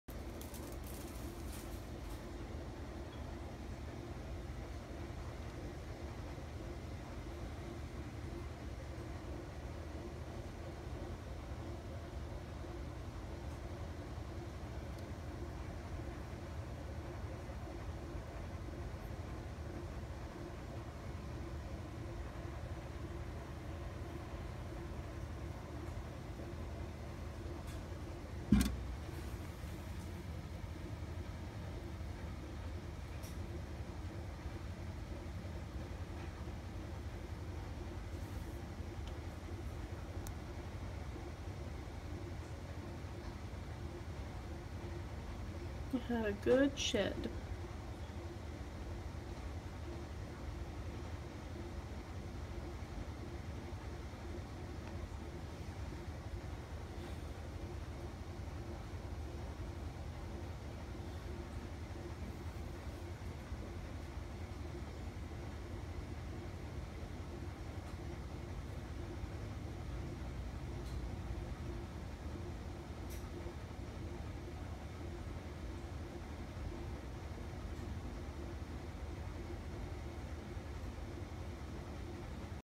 Baby ball python
My newest rescue pet. Super sweet baby. Just freshly shed his skin